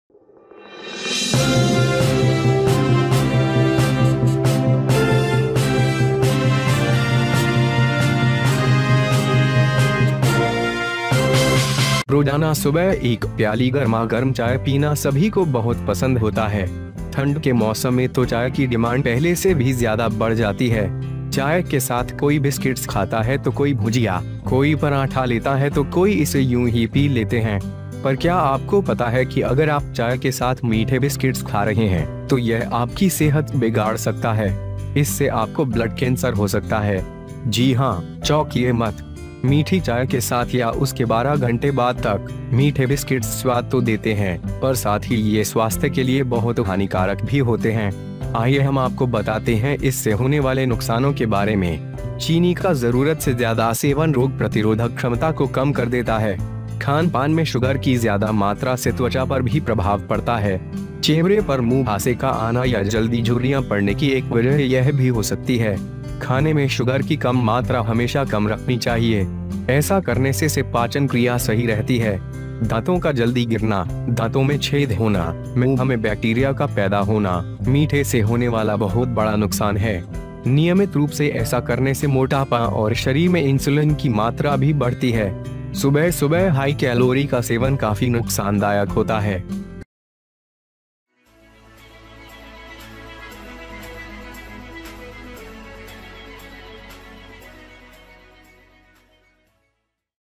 चाय पीने के बाद भूलकर भी ना खाएं ये चीज, वरना होगा कुछ ऐसा की...
चाय पीने के 12 घंटे बाद तक भूलकर भी ना खाएं ये चीज, वरना होगा कुछ ऐसा की...

Every morning, drinking a cup of hot tea everybody loves them very much. In cold weather, demand for tea increases even more than before. If there is a biscuits with tea, a bhujia, someone takes a paratha, then someone drink it.
But do you know that if you are eating sweet biscuits with tea then it can spoil your health. This can lead to blood cancer. Yes, do not interfere. Sweet biscuits taste with sweet tea or up to 12 hours, but at the same time it is also very harmful for health. Let us tell you about the losses caused by it.
Excessive intake of sugar reduces immunity to disease. The excessive amount of sugar in the diet is also affected by the skin. There may also be a reason for acne coming on the face or early wrinkles. Low amount of sugar in the food should always be kept low. By doing so the digestive system remains correct.
Tooth decay, tooth decay, bacterial growth in the mouth, there is a great loss due to sweetness. By doing this regularly, obesity and the amount of insulin in the body also increases. High calorie intake is very harmful in the morning.

HJ NEWS !!